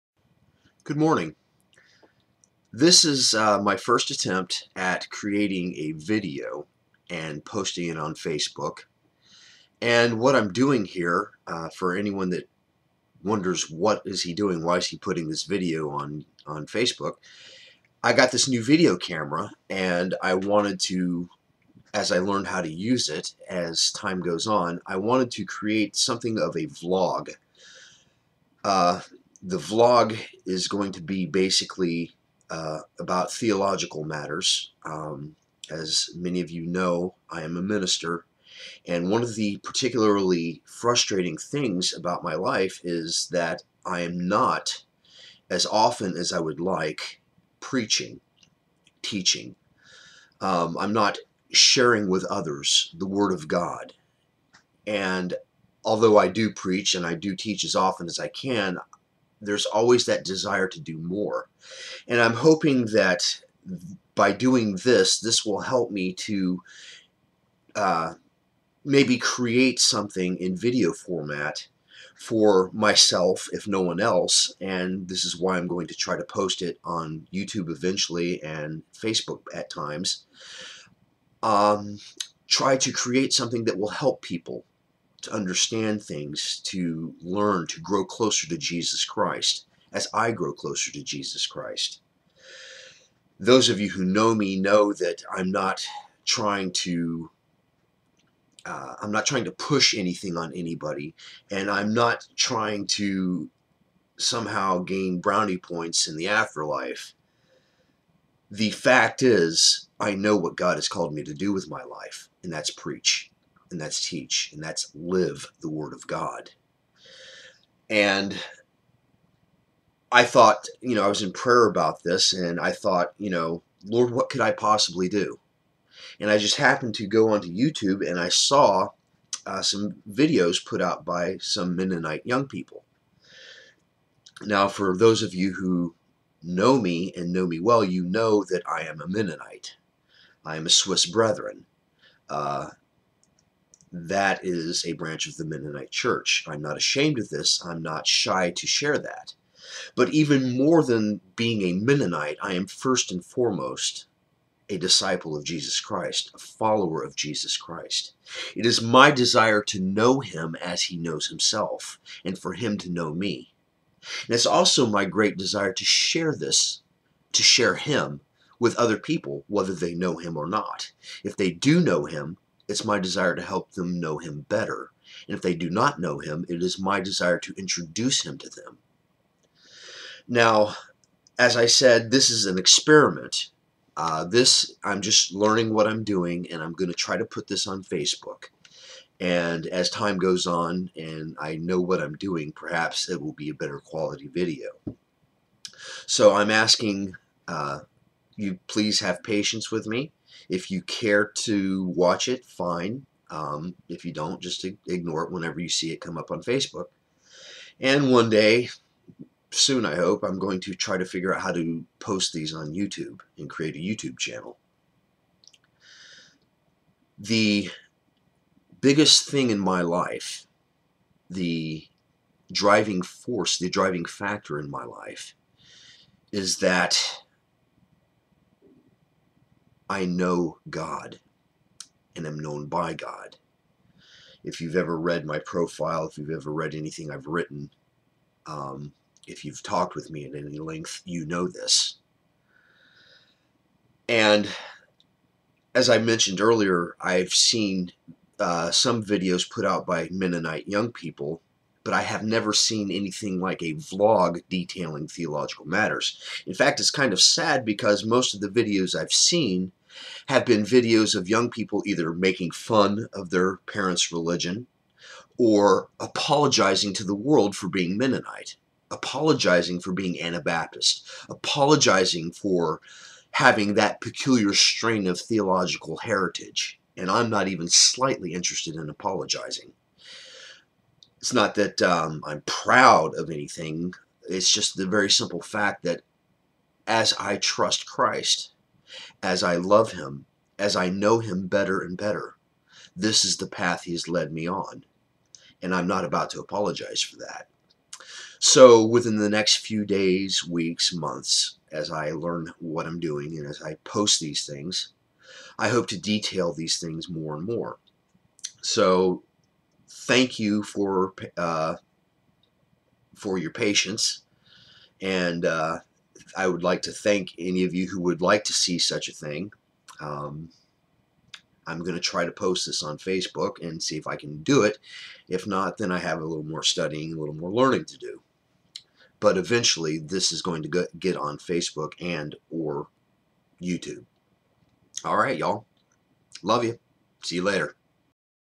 Introduction To My Vlog/Blog
Just a short video to introduce what I'm doing. My name is William Allen, and I'm trying to create a video log for myself, my friends, and anyone who might be interested.